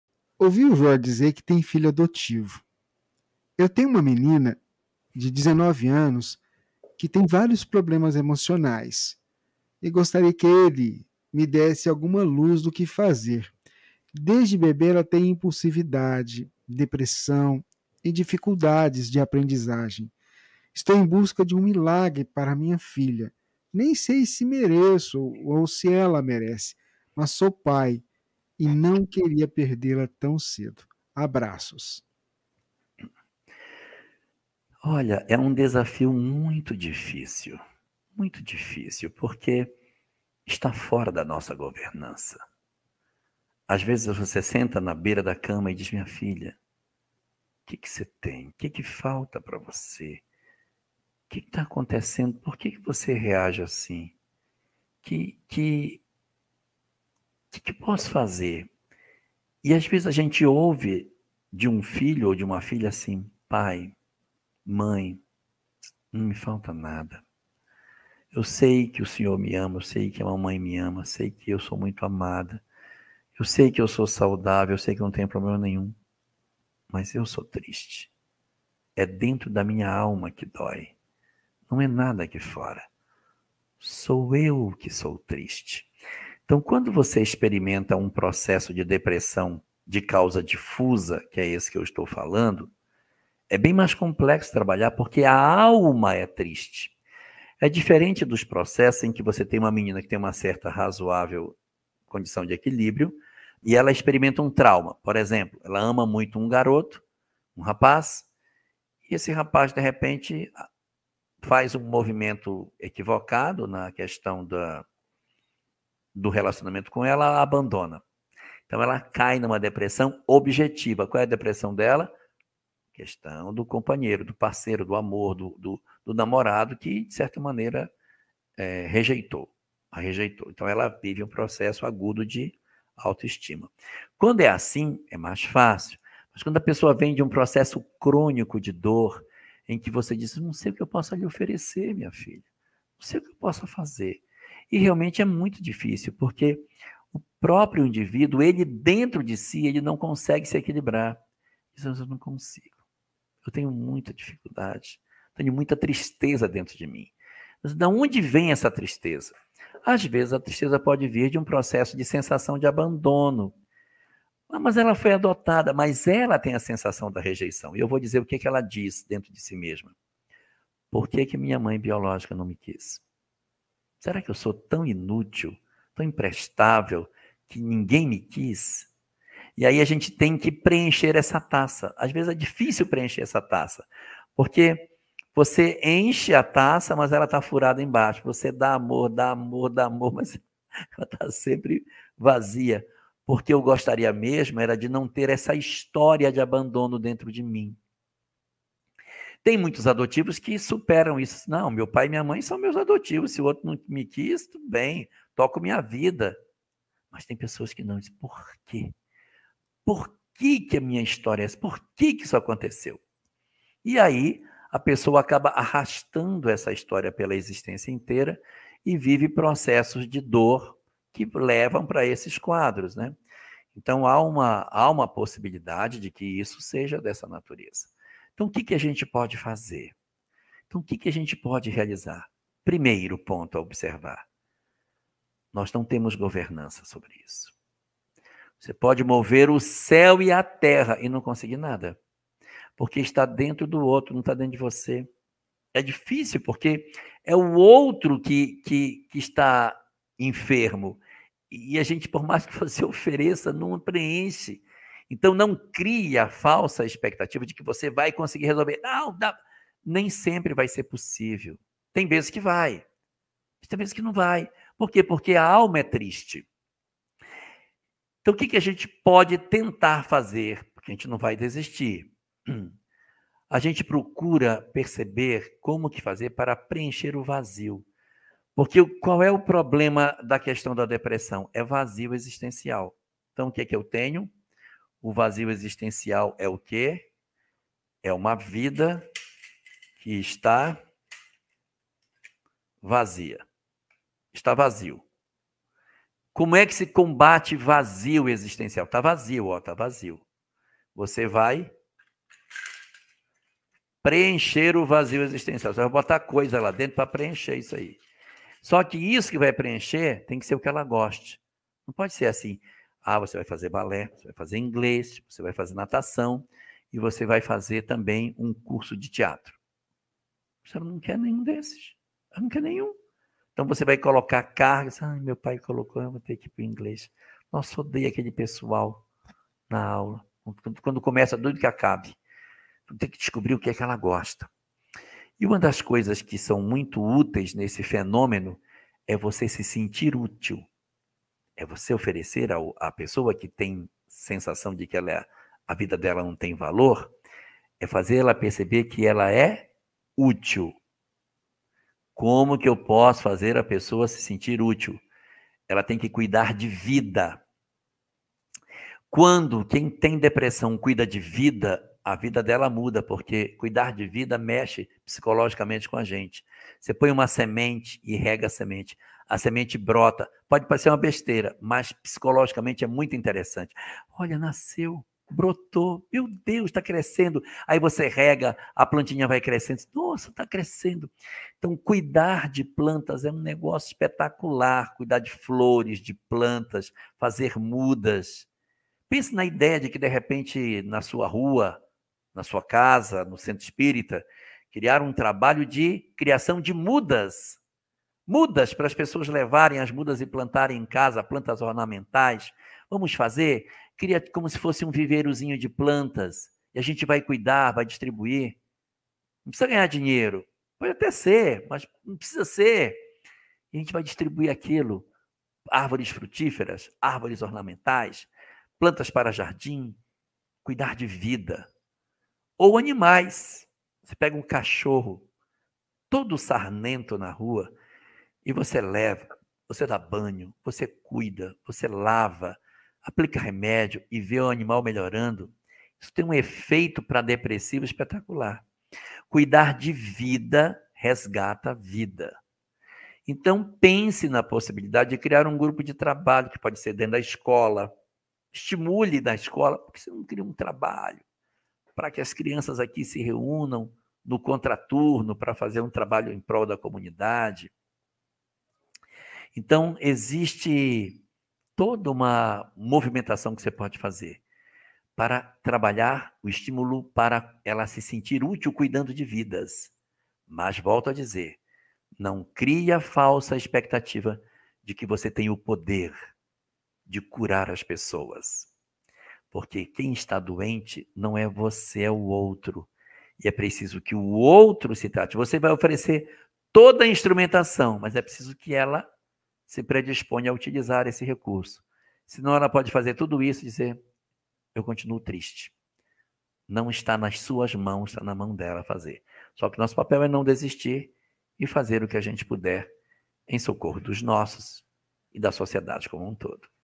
Como ajudar uma filha adotiva de 19 anos que tem vários problemas emocionais? Jorge Elarrat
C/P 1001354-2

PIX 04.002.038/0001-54

Gratidão!